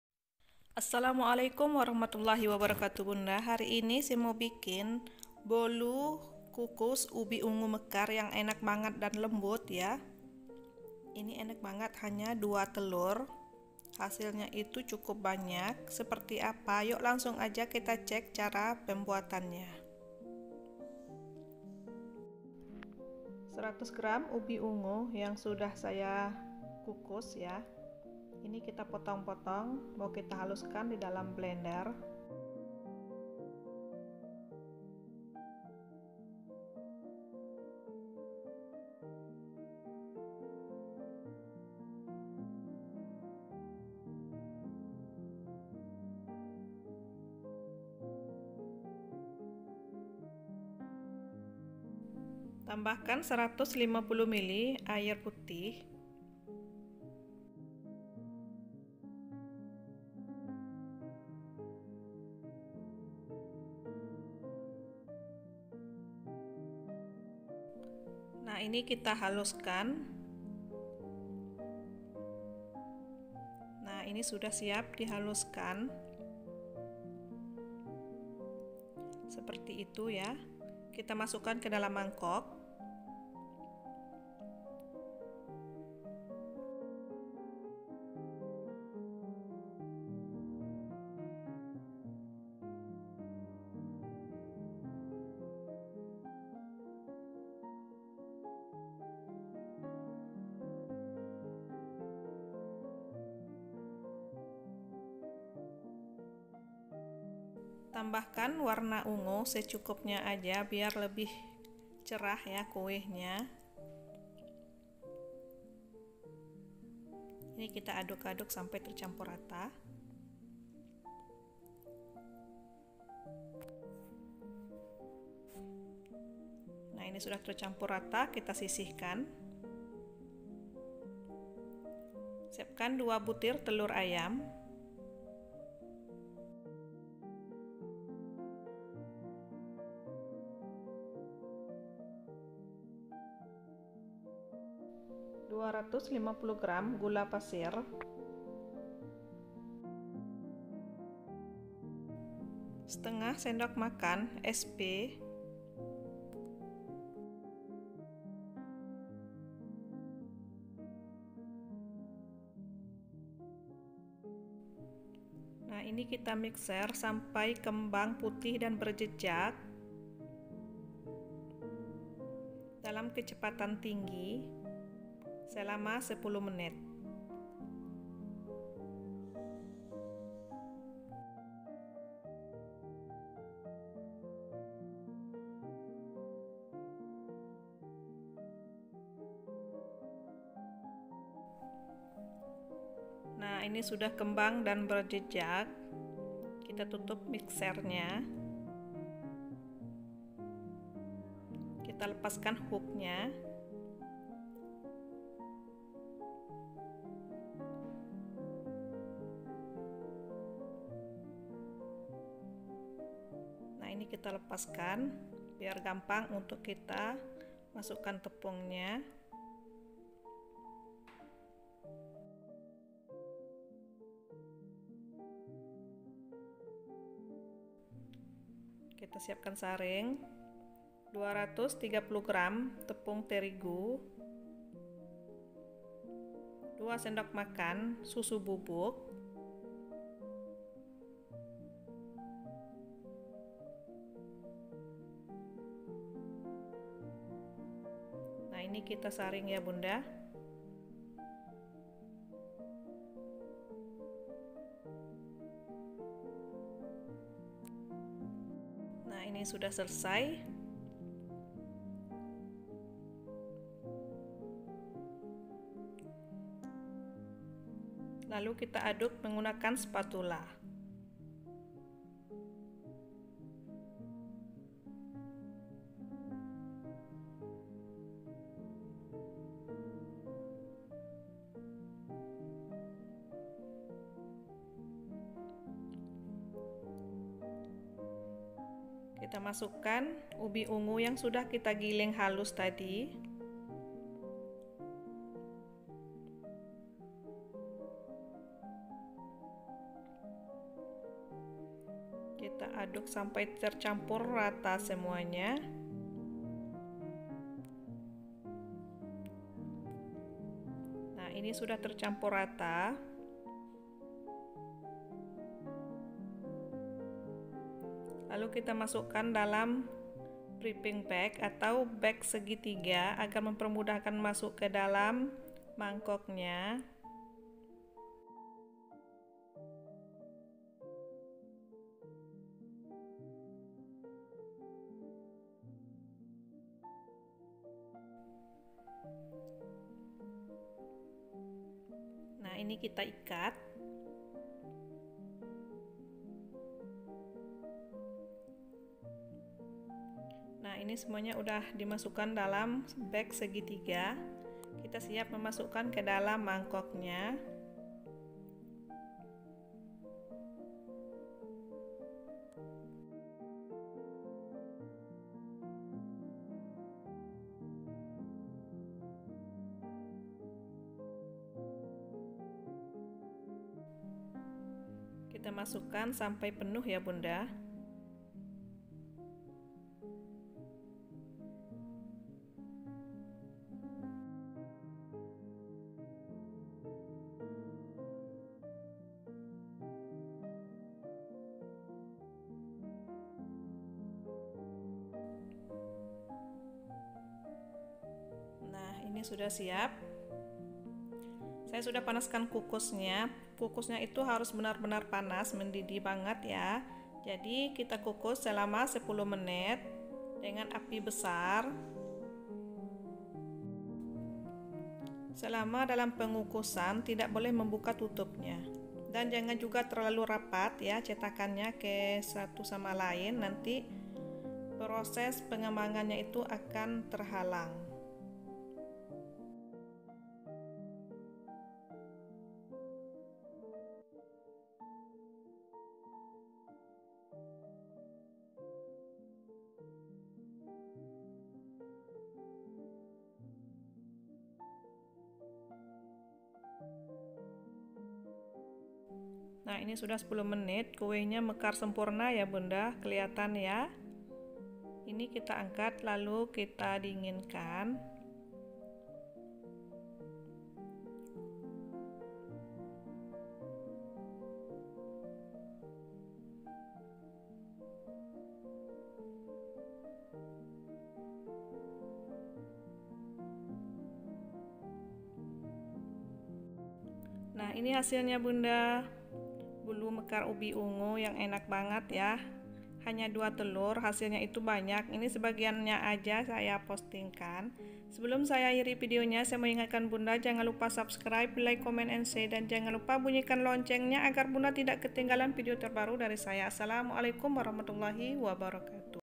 bolu kukus mekar ubi ungu hanya dua telur, super lembut anti gagal
bahan
100 gram ubi ungu sudah di kukus
150 mili air
pewarna ungu secukup nya aja
2 butir telur ayam
250 gram gula pasir
1/2 sdm sp
230 gram tepung terigu
2 sdm susu bubuk

cara membuat nya langsung cek di video nya, jangan di skip agar tidak salah paham

Terimakasih 😘😘😘